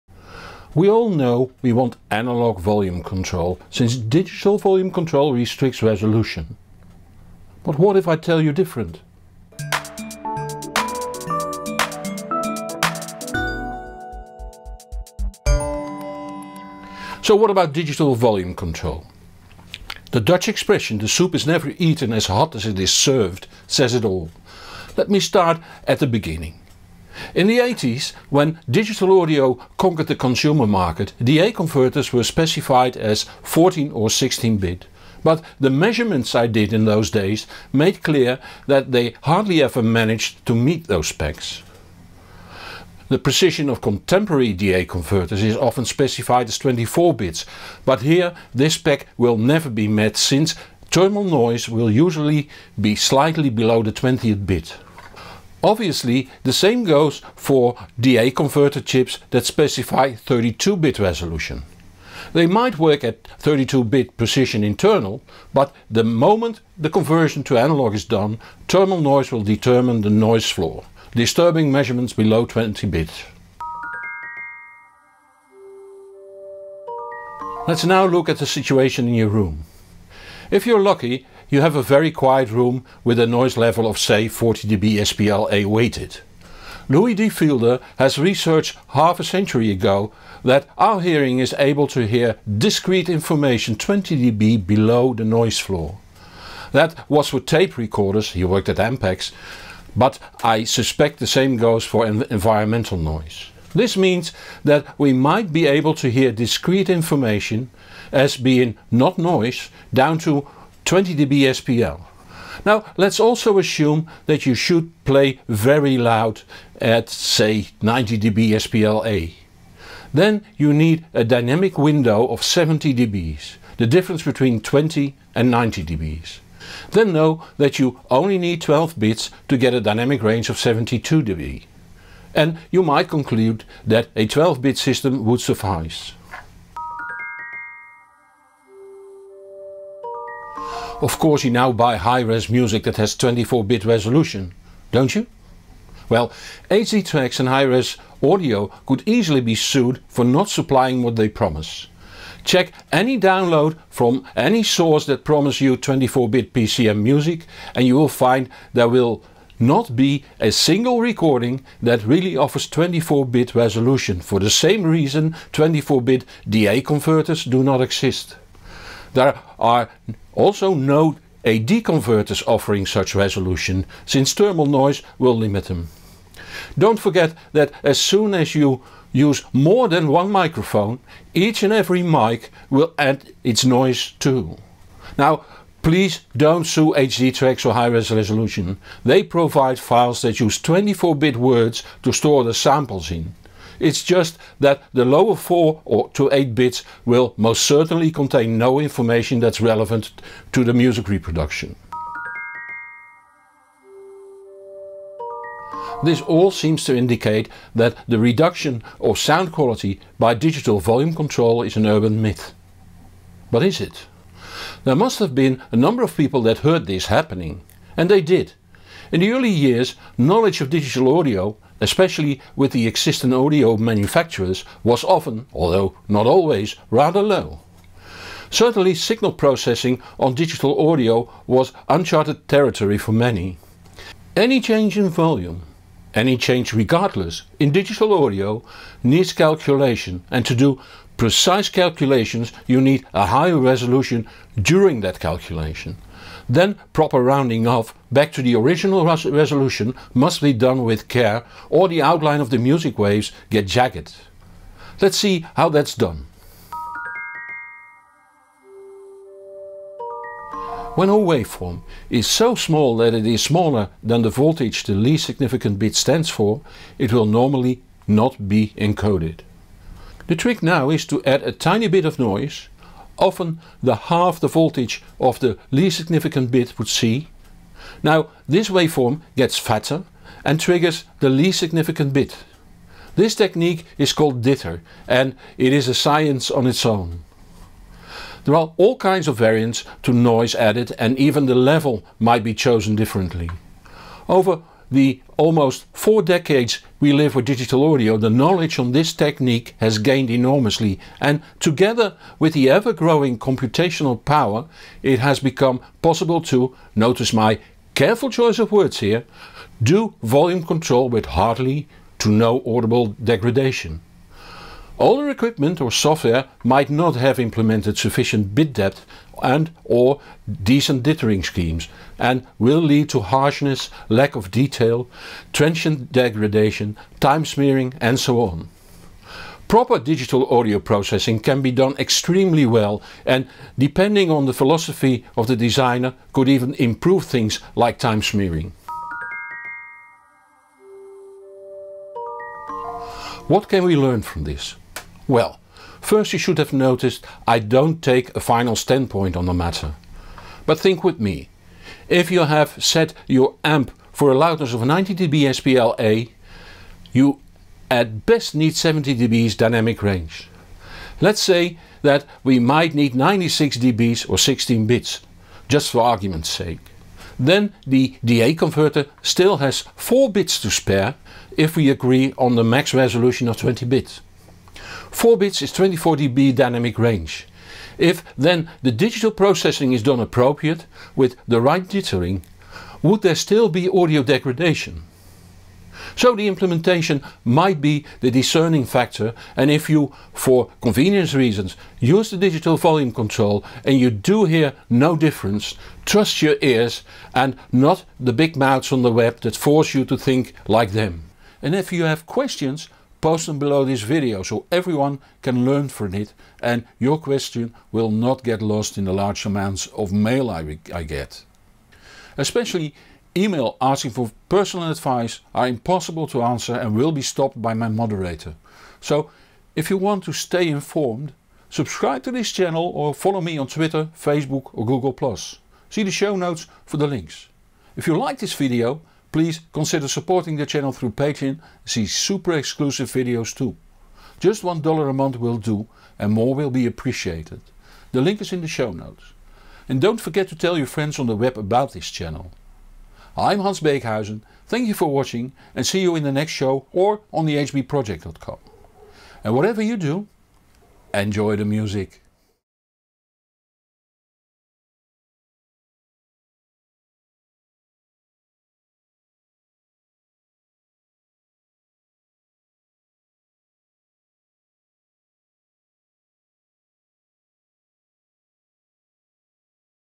Does digital volume control really suck?
We all know that we want analogue volume control since digital volume control restricts resolution. But what if I tell you different….